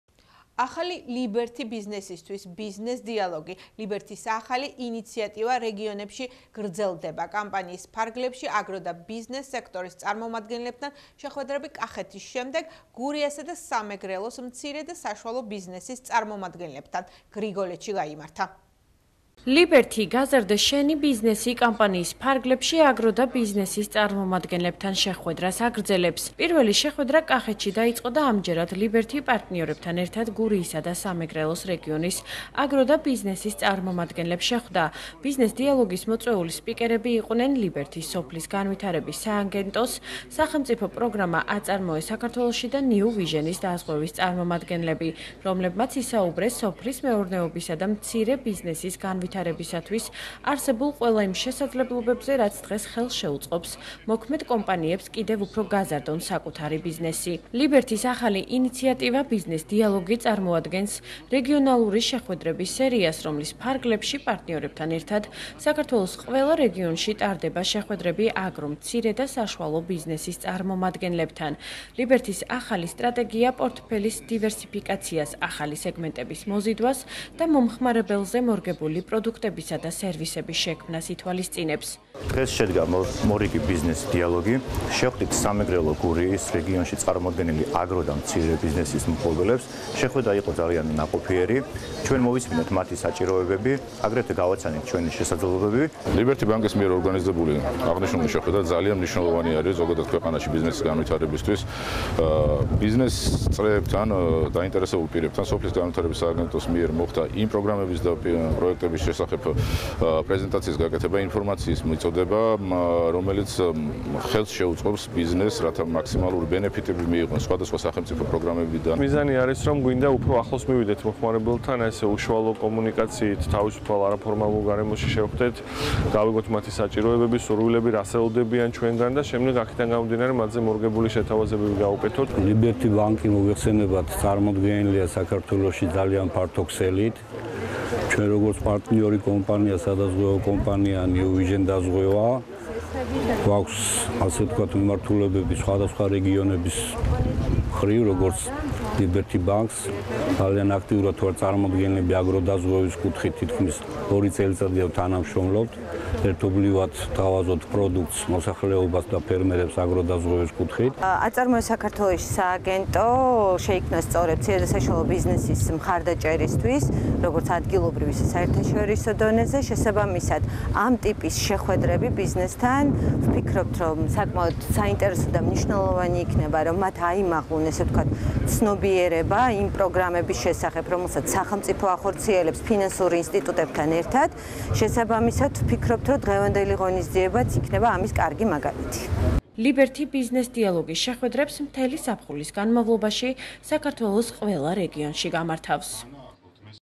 ახალი ლიბერთი ბიზნესისთვის
,,ბიზნეს დიალოგი“ -  ლიბერთის ახალი ინიციატივა რეგიონებში გრელდება. კამპანიის ფარგლებში აგრო და  ბიზნეს სექტორის წარმომადგენლებთან შეხვედრები კახეთის შემდეგ, გურიისა და სამეგრელოს მცირე  და საშუალო ბიზნესის წარმომადგენლებისთის გრიგოლთში გამართა.
 შეხვედრის მონაწილეებს ინფორმაცია სოფლის მეურნეობისა და მცირე ბიზნესის განვითარებისთვის არსებულ ყველა იმ შესაძლებლობებზე მიაწოდეს, რაც დღეს ხელს შეუწყობს მოქმედ კომპანიებს კიდევ უფრო გაზარდონ საკუთარი ბიზნესი.
ლიბერთი ,,ბიზნეს დიალოგის’ შეხვედრებს,  მთელი ზაფხულის განმავლობაში  საქართველოს ყველა რეგიონში გამართავს.